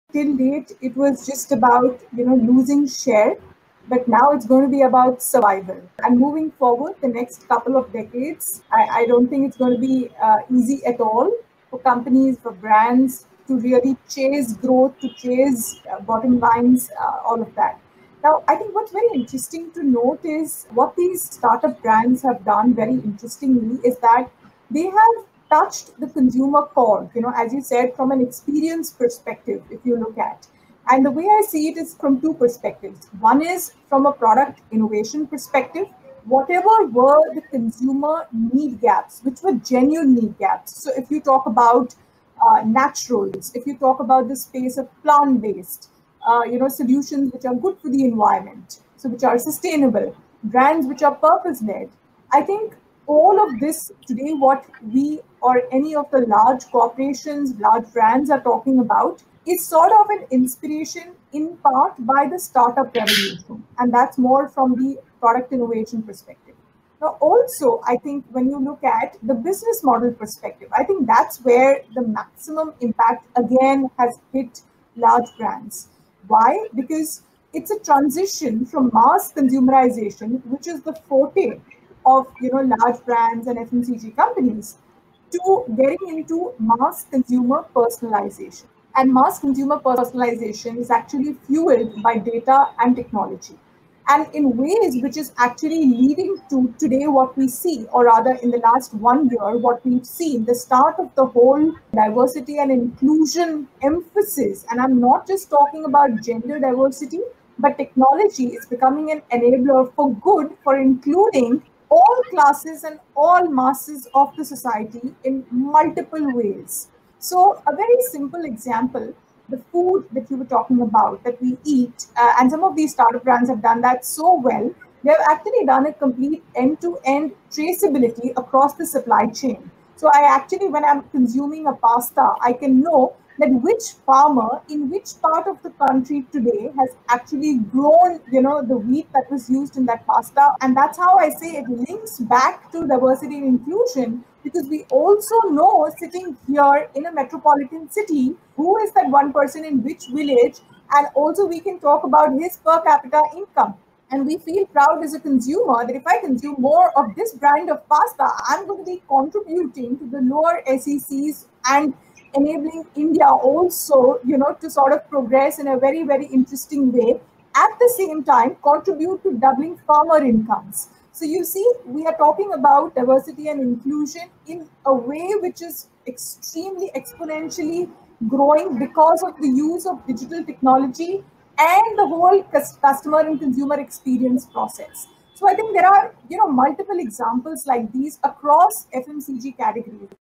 ChattyBytes | Amrita Asrani on Inclusion & Diversity in Digital Transformation in FMCG & Retail
In this excerpt, Amrita Asrani, Global Brand Marketing Directors, Fabric Care, Reckitt & Benckiser talks about the other aspect of Digital Transformation - Inclusion and Diversity.